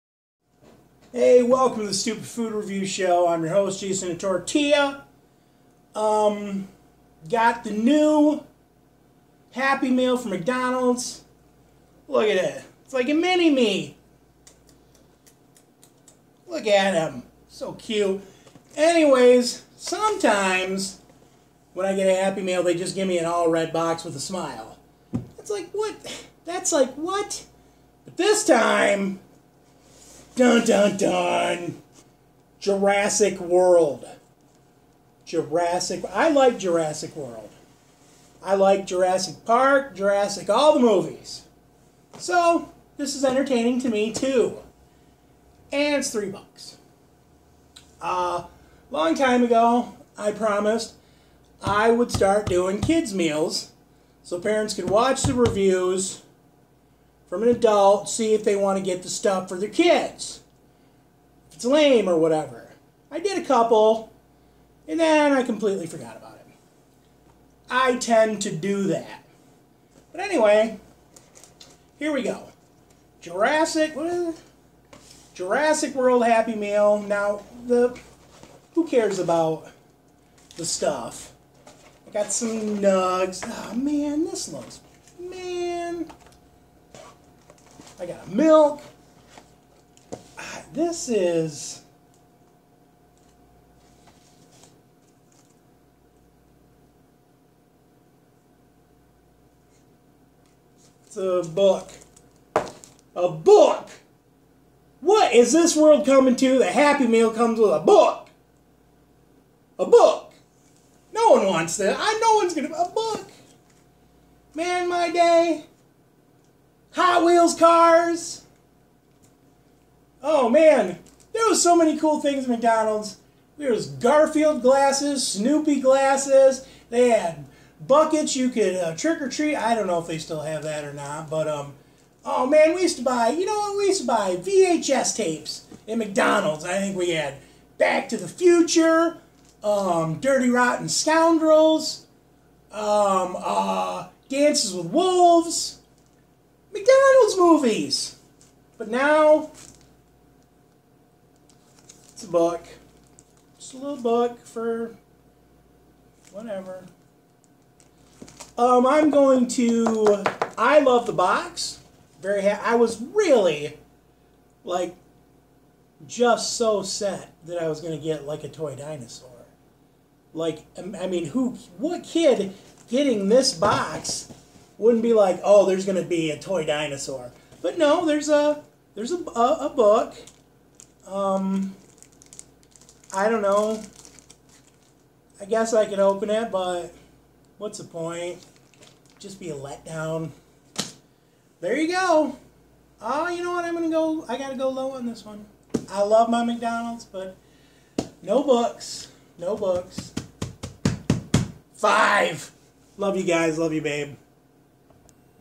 McDonald's NEW Jurassic World Happy Meal! 🐊🍔🍟
Jurassic World Happy Meal @ McDonald's. Comes with a activity book.